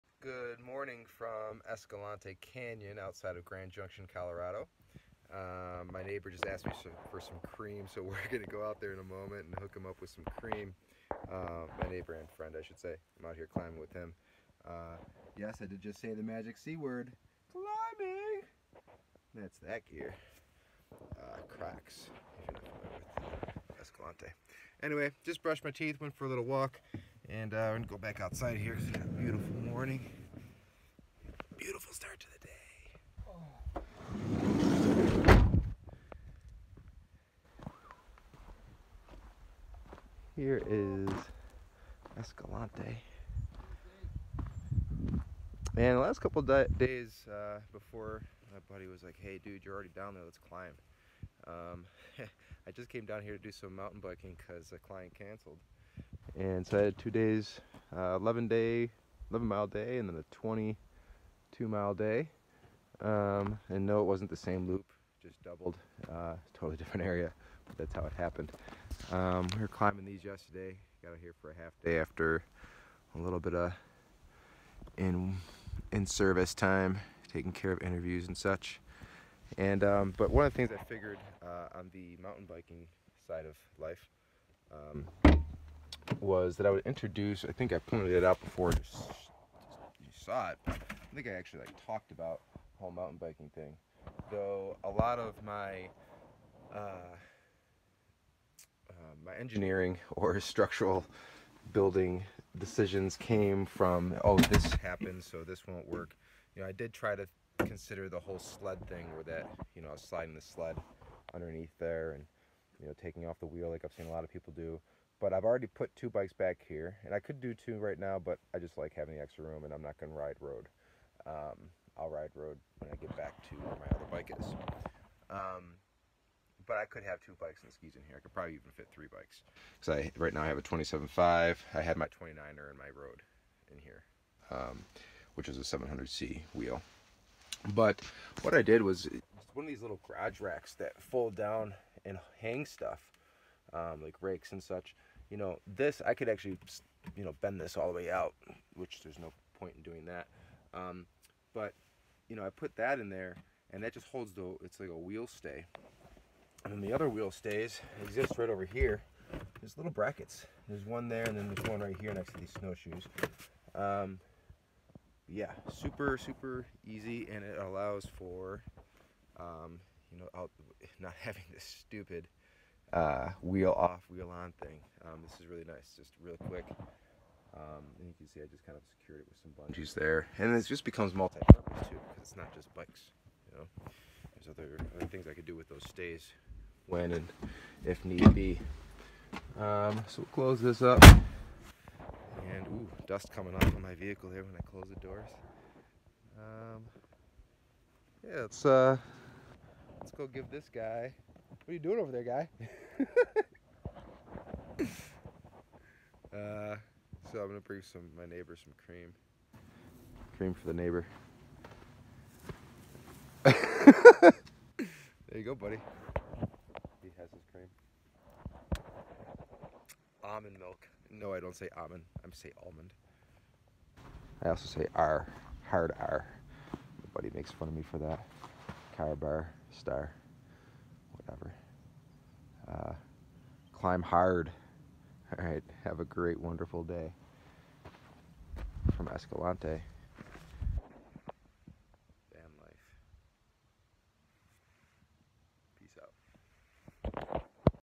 Greetings from Escalante!
Goodmorning! How do I store my bikes?? And, who needs creamer?!